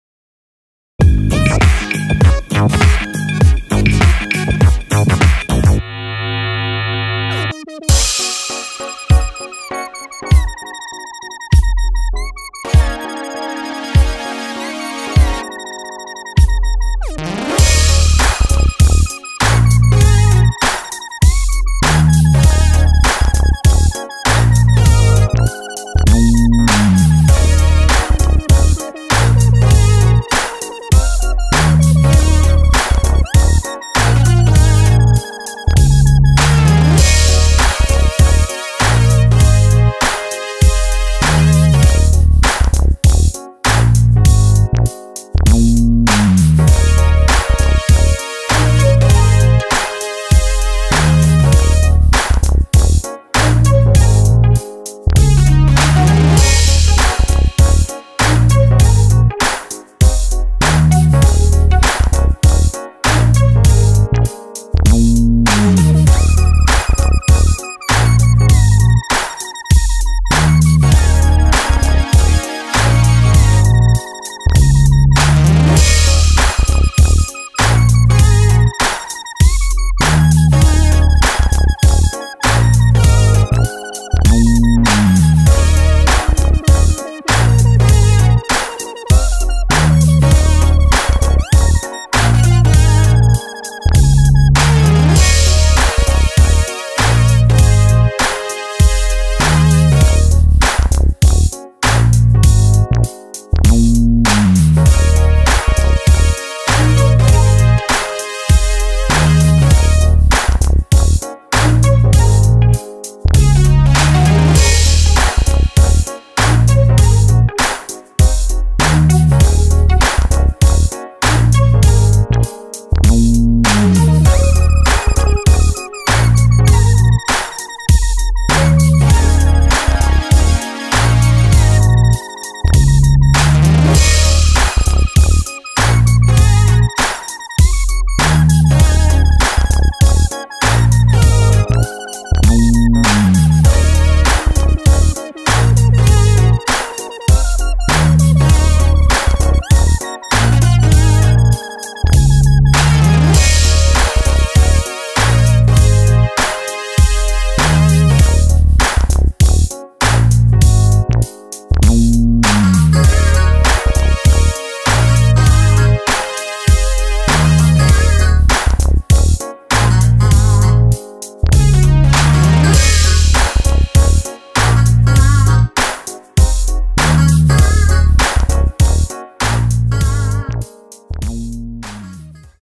Eachine QX65 FPV Drone - Woodland Benches
I slowly work myself up into the most precise flying I've ever done! definitely feeling it more and more with every flight now 🔥